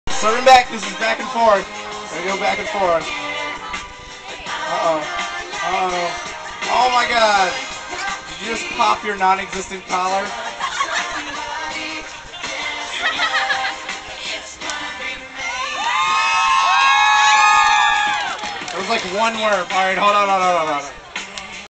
Dancing to N*Sync at Warped Tour
2 random guys having a dance off at warped tour